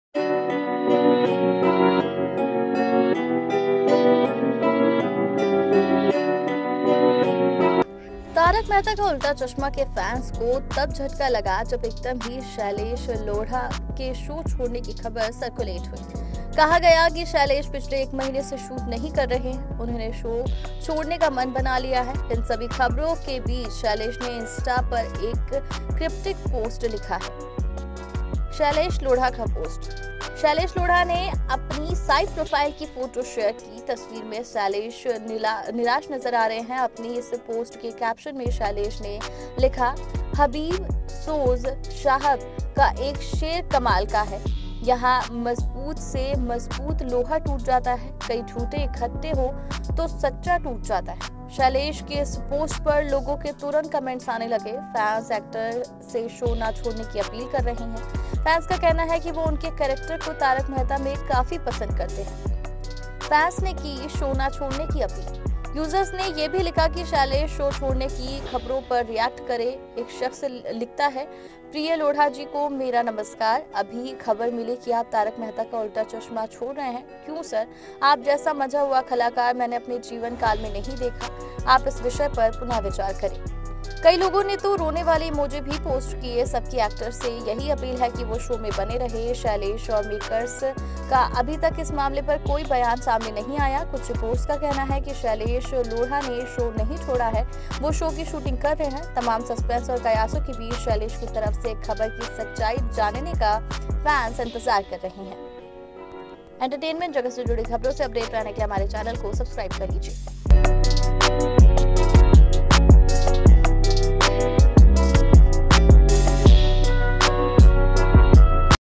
Shailesh Lodha Speak FIRST Time After Quit 'Taarak Mehta Ka Ooltah Chashma' Show !
Taarak Mehta Cryptic Post Amid Rumours Of His Exit From The TMKOC Show | Shailesh Lodha Speak FIRST Time After Quit 'Taarak Mehta Ka Ooltah Chashma' Show